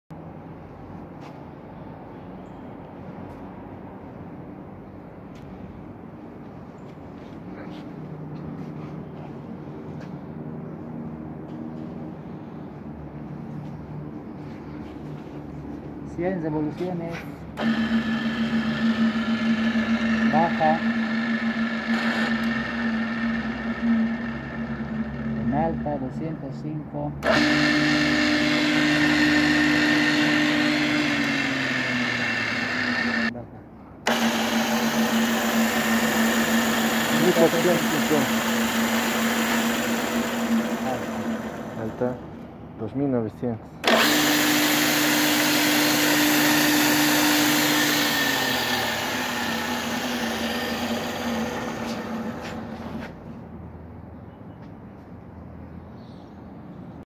2146 TALADRO DE COLUMNA ARBOGA
Modelo: G 2508
Velocidad del husillo: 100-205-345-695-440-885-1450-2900 Rpm.
Carrera del husillo: 11 cm.
Cono morse: 3
Potencia del motor: 1 Hp.
Alimentación: 380, 3f.
Industria: Suecia.